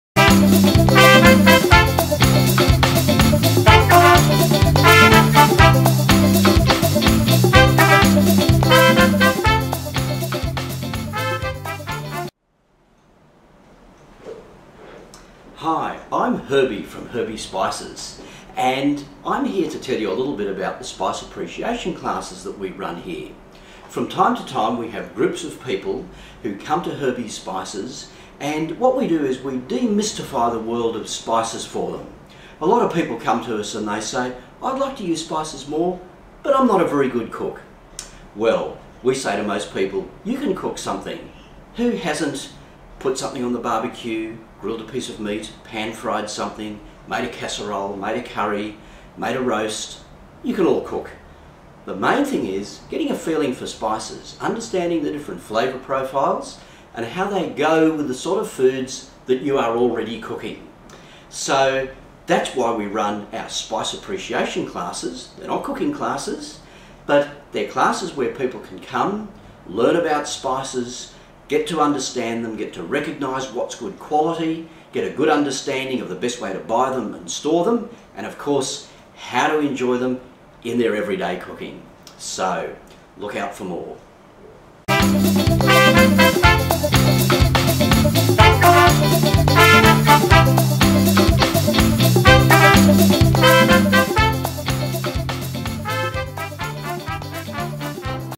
Herbies Spice Classes
Herbie talks about the spice classes  that he runs at his Rozelle shop in Sydney